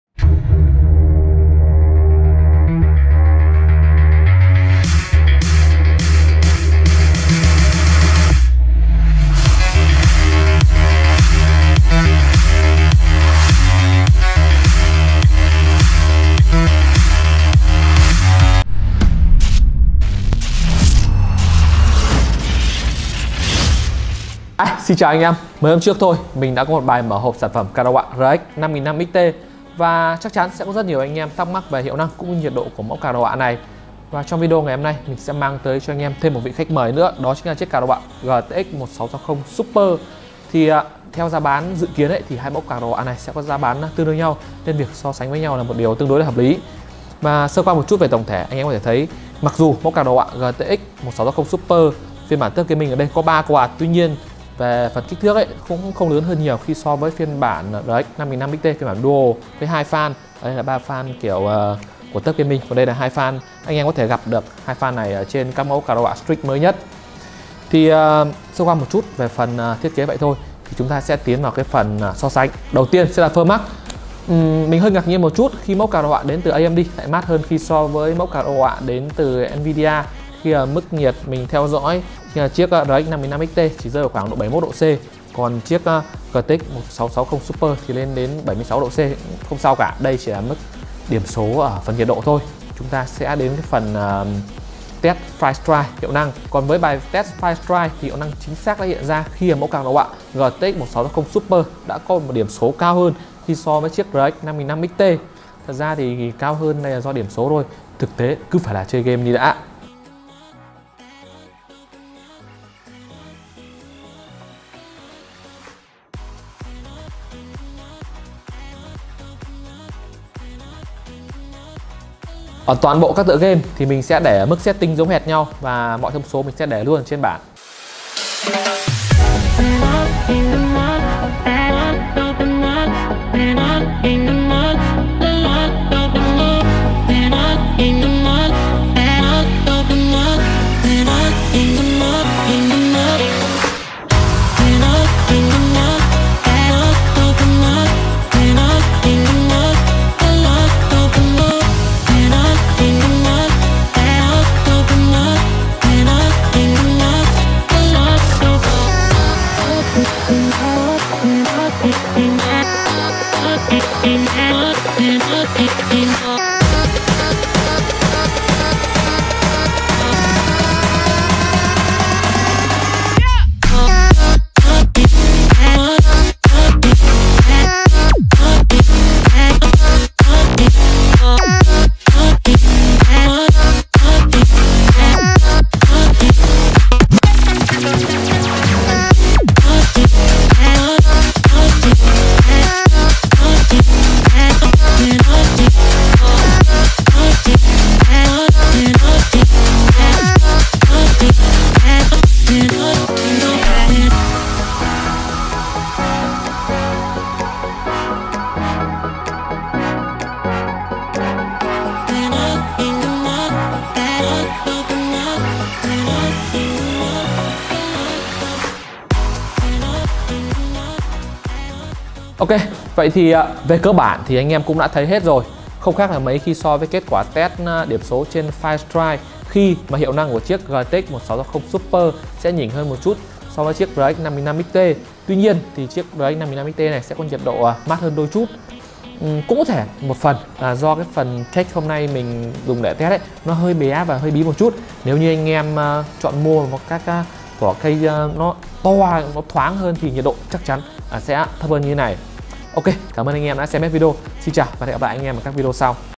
So sánh hiệu năng GTX 1660 Super và RX 5500XT | An Phat PC
Sau sự ra mắt của RX 5500XT thì chắc chắn anh em sẽ thắc mắc về hiệu năng của mẫu card đồ họa này. Vậy thì hôm nay AnPhat sẽ gửi đến cho anh em bài so sánh hiệu năng của Rx 5500XT với card đồ họa GTX 1660 Super để có cái nhìn trực quan nhất về hiệu năng của 2 mẫu card đồ họa này.

📌Và đừng quên anh em có các điều gì thắc mắc đừng quên để lại COMMENT ở phía dưới nhé.

SIÊU THỊ MÁY TÍNH AN PHÁT